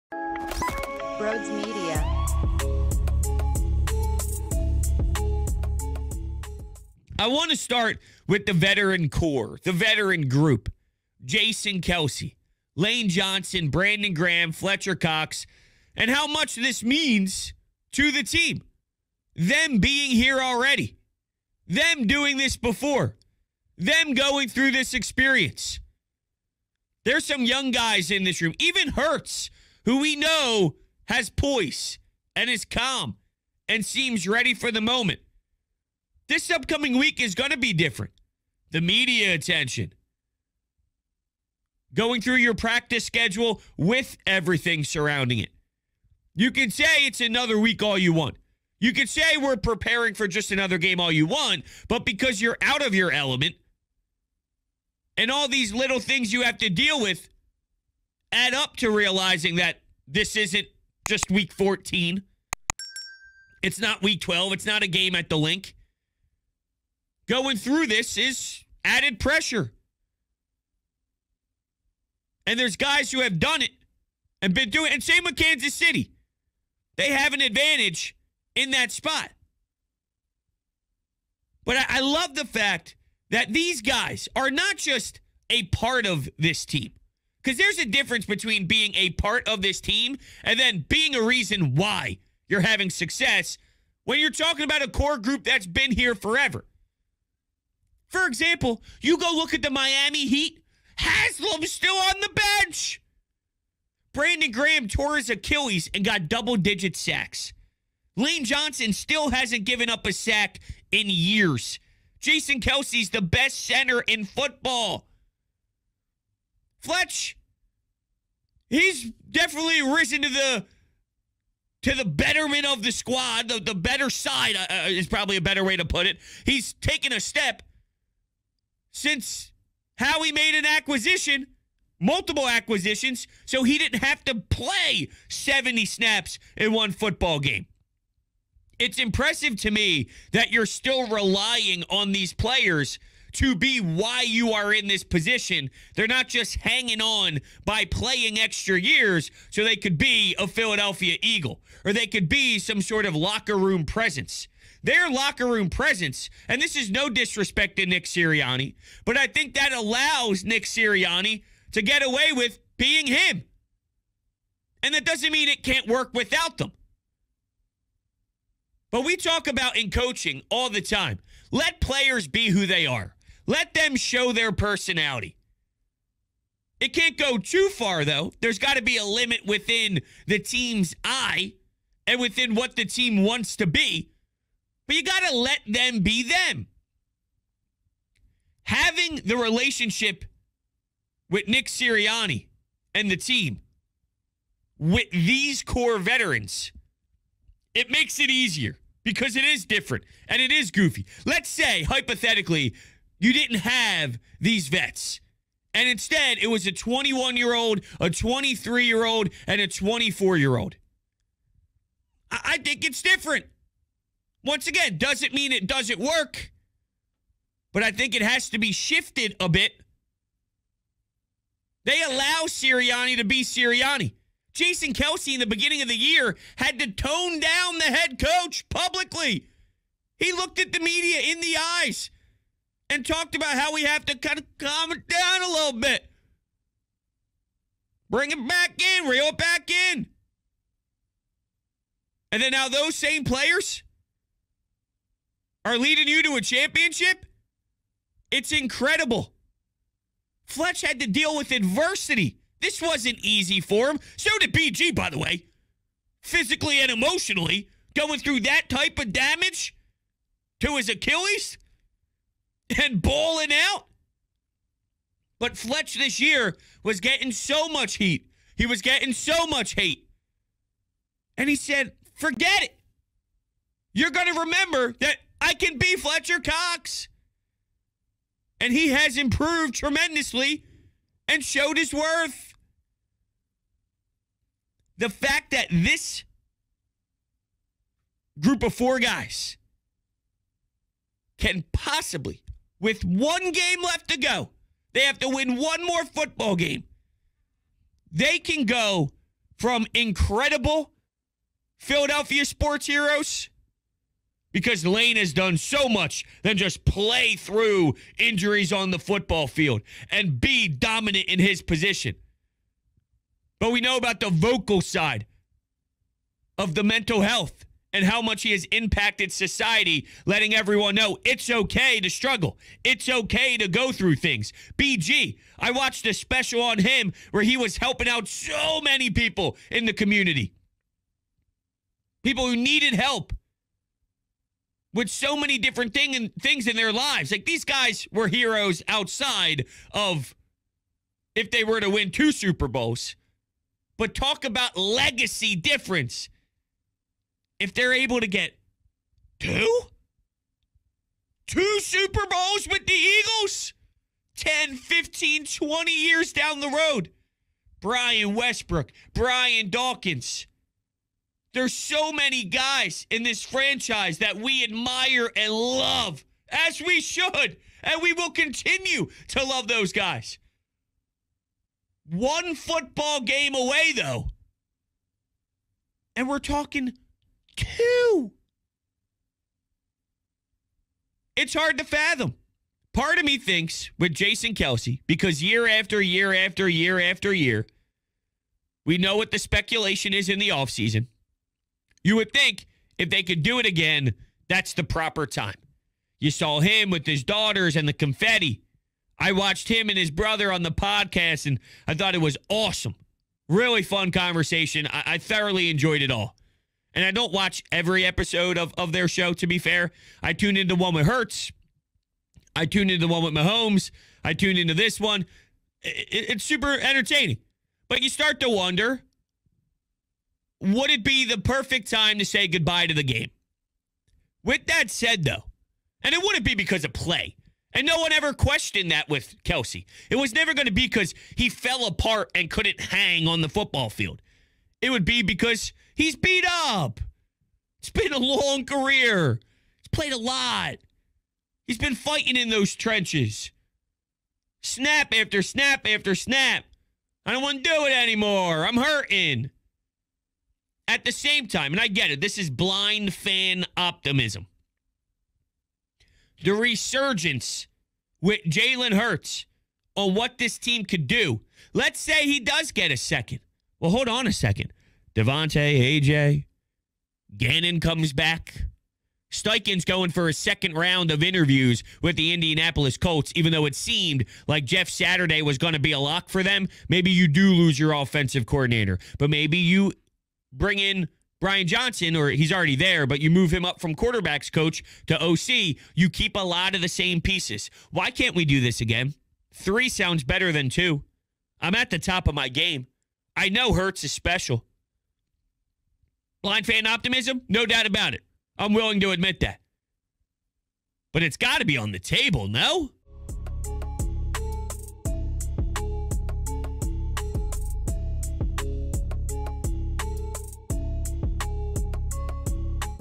EAGLES VETERAN CORE CAN ADD TO THEIR LEGACY WITH ANOTHER SUPERBOWL WIN!
The Philadelphia Eagles are on their way to the Super Bowl. What did the veterans do to help get there? Also, Jerry Jones thinks the Birds are in a bad spot?!

This will save you $20 on your total purchase!

Twitter & Instagram: @Brodes81 and @BrodesMedia